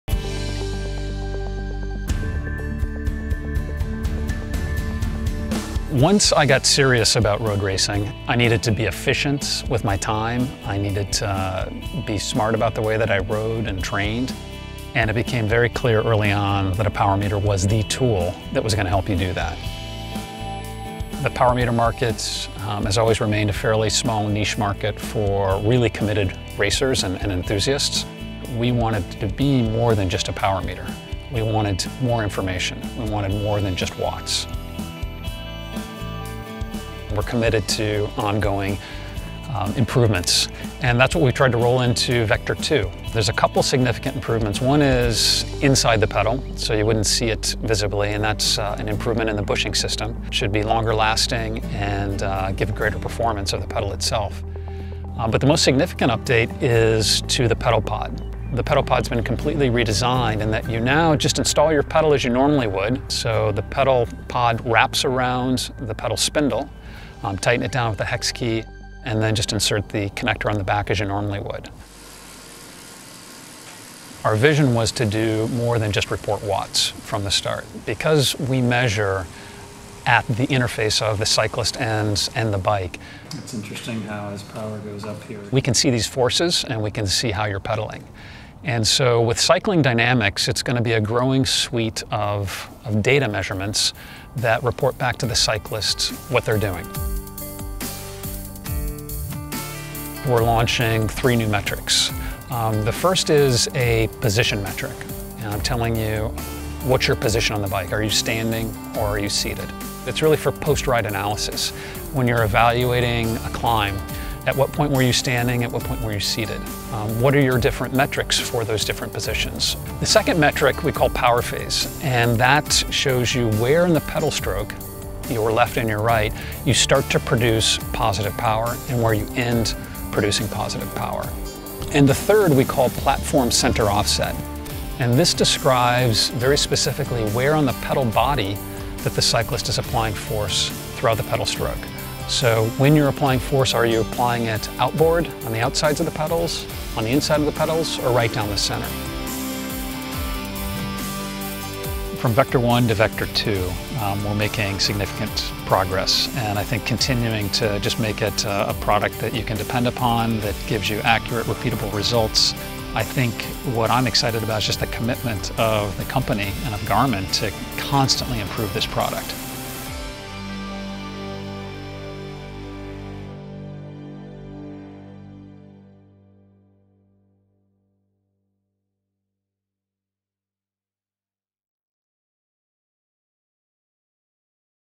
Garmin Vector 2: More Than Just Power
Garmin brings more than just power to the people with Vector 2, the dual-sensing pedal-based cycling power meter. We’ll explain why a power meter is the best way for really committed cyclists and enthusiasts to measure how their bodies respond to training load.

To learn more about our latest Vector™ power meter bike pedals look